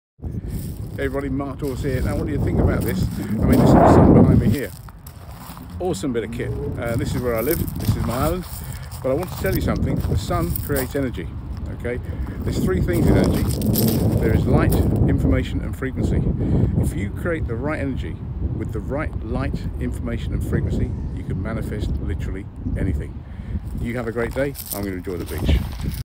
Energy is Light, Frequency and Information
Our body's are made up of organs.

Organs are made up of cells (approximately 50-60 Trillion cells)

Cells are made up of atoms and subatomic particles, that pop in an out of existence so are never permanent.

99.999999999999% (recurring) of an atom/subatomic particle is empty space.

Take all of that empty space out of every atom in your body and what would be left of you (in the physical sense) would be nothing bigger than a grain of sand.

Take all of that empty space out of every atom in every human being on earth (all nearly 8 billion of us), and the remaining physical matter left behind would be no bigger than a tennis ball or an apple.

The empty space however is not empty space. It is energy.

An energy contains; light, frequency and information.

So, make sure your energy is positive and not negative.

Operate at the right frequency to manifest whatever your goals are.

See your goal achieved in the light of your mind.